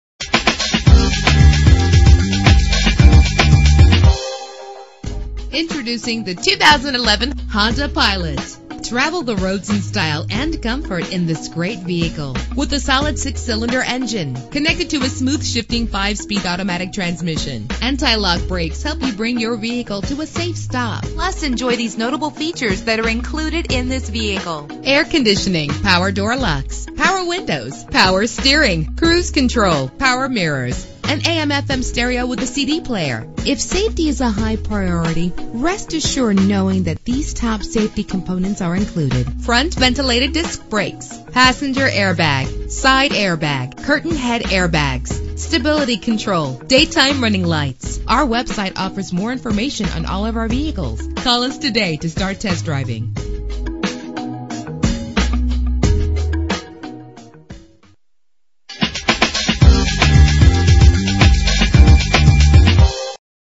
2011 Honda Pilot - Savannah GA
Year: 2011
Make: Honda
Model: Pilot
Trim: LX
Engine: 3.5 liter 6 cylinder 24 valve
Transmission: 5-Speed Automatic
Color: White
Mileage: 465
Address:
10300 Abercorn St